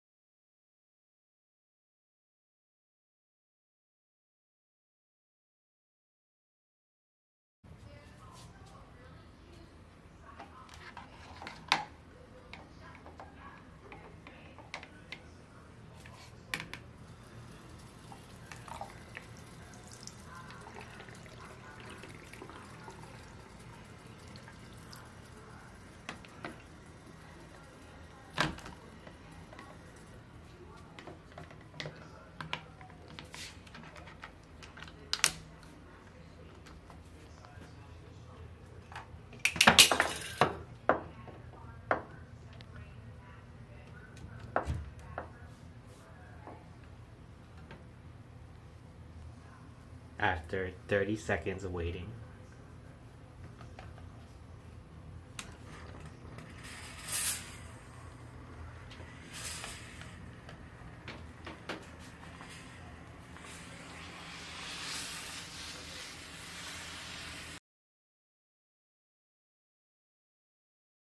How to steam out wrinkled smooth shirt with Glamfields clothes steamer|2018
Save money get hous & beauty product with code.
The best clothes steamers and garment steamers on Amazon, according to hyperenthusiastic reviewers, including the best handheld garment ...
Ironing is a pain, but wrinkles look sloppy. To get wrinkles out of your clothes without an iron, you need a steamer.